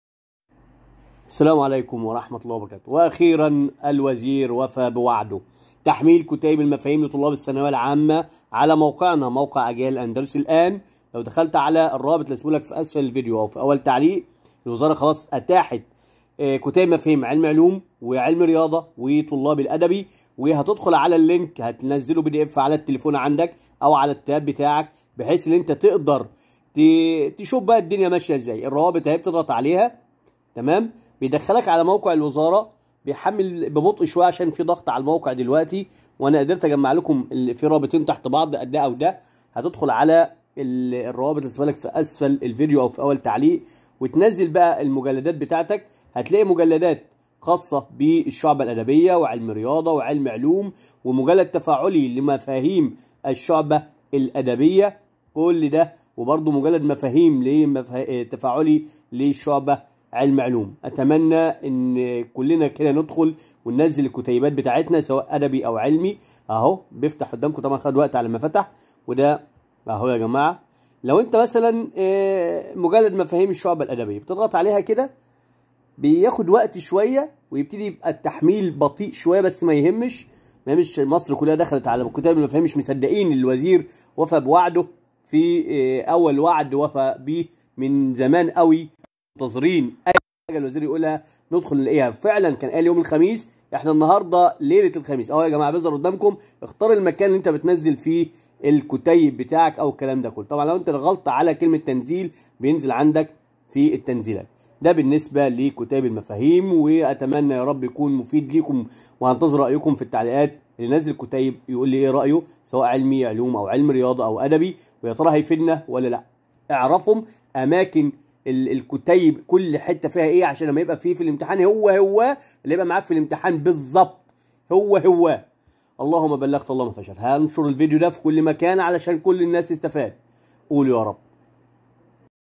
عاجل الان | يالا نحمل كتيب المفاهيم لطلاب الثانويه العامه علمي وادبي | هننجح ونحقق احلامنا
مجلد مفاهيم أدبي
نظرا الضغط الهائل على موقع الوزارة يمكن استخدام ال QR Code هنا لتحميل مجلد مفاهيم "أدبي" من موقع بديل.
مجلد مفاهيم علمي علوم
نظرا الضغط الهائل على موقع الوزارة يمكن استخدام ال QR Code هنا لتحميل مجلد مفاهيم "علمي علوم" من موقع بديل.
مجلد مفاهيم علمي رياضيات
نظرا الضغط الهائل على موقع الوزارة يمكن استخدام ال QR Code هنا لتحميل مجلد مفاهيم "علمي رياضيات" من موقع بديل.
تتيح وزارة التربية والتعليم والتعليم الفني، ٣ مجلدات مفاهيم" رقميا، اليوم الخميس، لطلاب الثالث الثانوي.

سيكون المجلد الأول لطلبة [علمي علوم]، والثاني لطلبة [علمي رياضه] والثالث لطلبة [أدبي].

واعلن وزير التعليم، أن تتاح المجلدات بصورتين: PDF + Ebook كى يتعرف الطلاب عليها قبل الامتحان بشهر كامل وسوف تطبعها وتوزعها الوزارة اثناء الامتحانات.

كما أعلن وزير التعليم، ان هذه النسخ الرقمية تغطي المناهج بأكملها وباللغات العربية والانجليزية والفرنسية للعلوم والرياضيات، وتابع ان هى نفس النسخ التي سوف يتم طباعتها وإتاحتها للطلاب اثناء الامتحانات.
لطلاب الصف الثالث الثانوي
يمكنك الوصول إلى النسخة الرقمية من "مجلدات المفاهيم" الخاصة بالشعب علمي/علوم، علمي /رياضيات وأدبي
وتشمل المناهج بأكملها باللغات العربية والانجليزية والفرنسية.